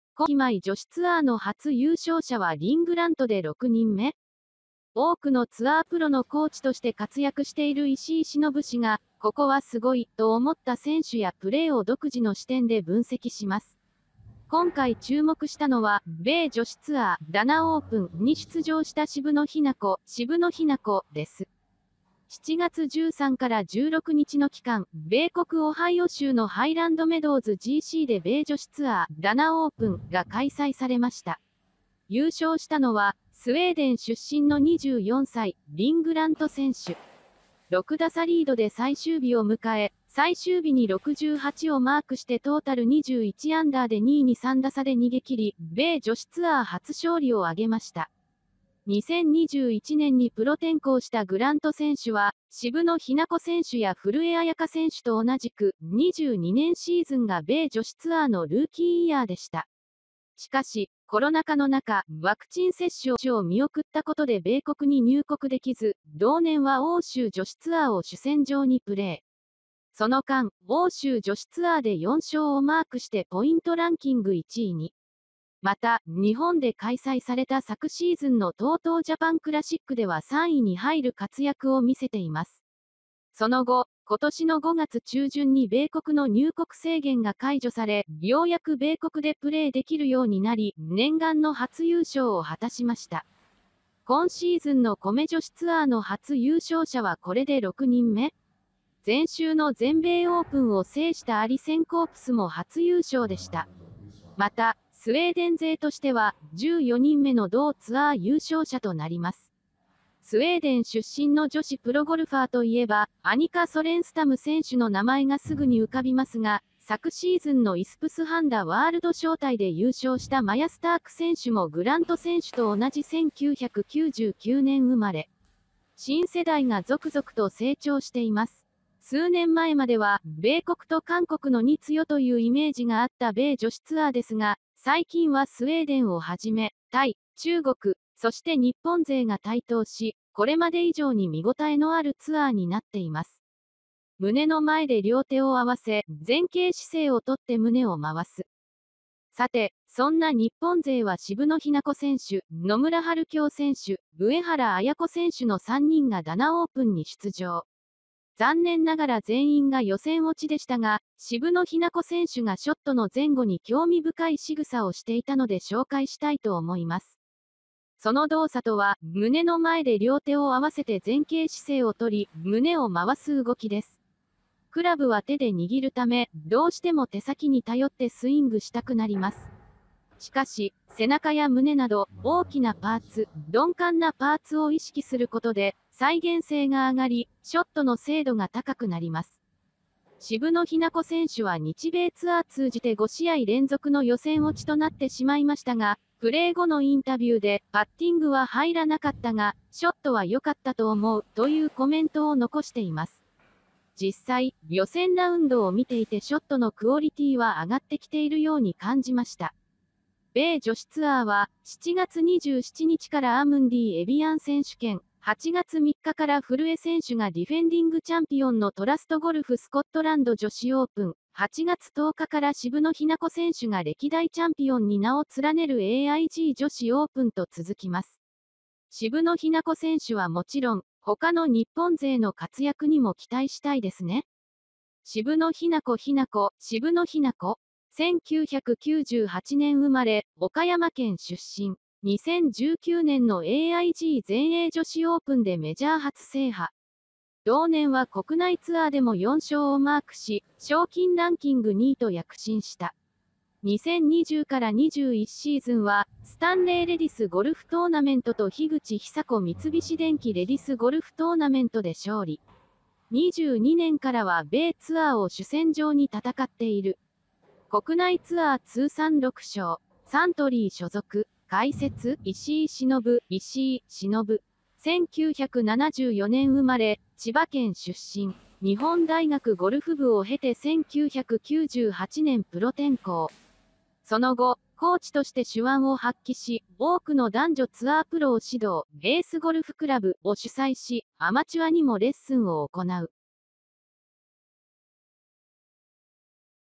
手打ち防止にも効果アリ！ 渋野日向子が取り入れる“鈍感なパーツ”を意識できる仕草とは？
今季米女子ツアーの初優勝者はリン・グラントで6人目
多くのツアープロのコーチとして活躍している石井忍氏が、“ここはスゴイ”と思った選手やプレーを独自の視点で分析します。今回注目したのは、米女子ツアー「Danaオープン」に出場した渋野日向子（しぶの・ひなこ）です。

7月13～16日の期間、米国オハイオ州のハイランドメドウズGCで米女子ツアー「Danaオープン」が開催されました。優勝したのは、スウェーデン出身の24歳、リン・グラント選手。6打差リードで最終日を迎え、最終日に68をマークしてトータル21アンダーで2位に3打差で逃げ切り、米女子ツアー初勝利を挙げました。

　2021年にプロ転向したグラント選手は、渋野日向子選手や古江彩佳選手と同じく、22年シーズンが米女子ツアーのルーキーイヤーでした。しかし、コロナ禍の中、ワクチン接種を見送ったことで米国に入国できず、同年は欧州女子ツアーを主戦場にプレー。その間、欧州女子ツアーで4勝をマークしてポイントランキング1位に。また、日本で開催された昨シーズンのTOTOジャパンクラシックでは3位に入る活躍を見せています。その後、今年の5月中旬に米国の入国制限が解除され、ようやく米国でプレーできるようになり、念願の初優勝を果たしました。